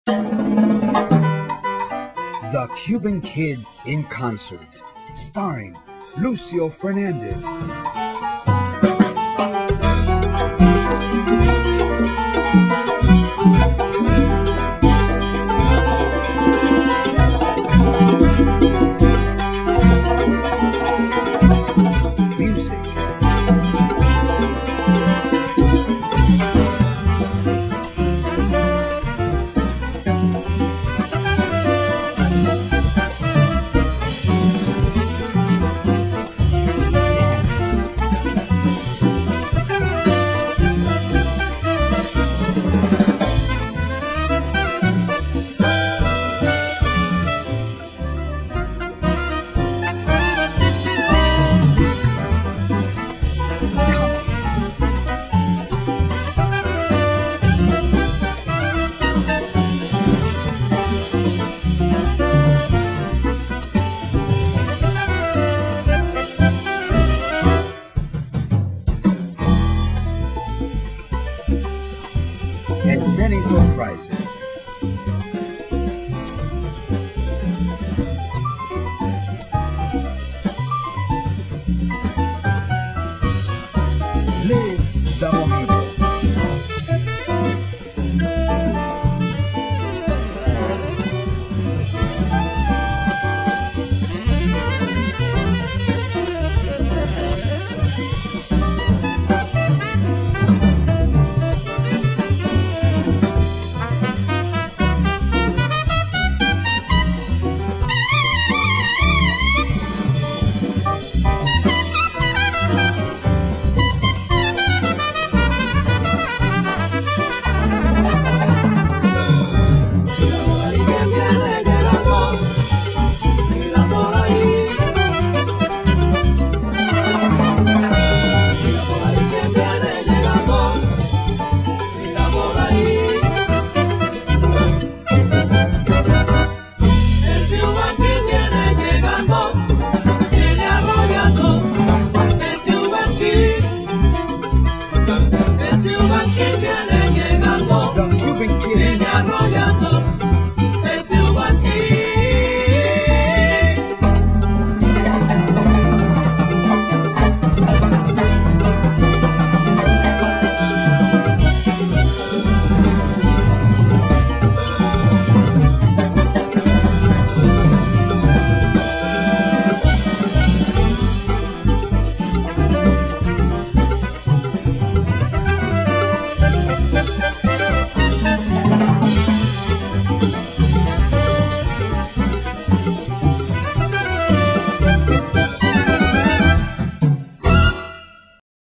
Lucio Fernandez "The Cuban Kid" Demo
The Cuban Kid in concert starring Lucio Fernandez and featuring the TapOlé Dance Company & The Cuban Kid Orchestra.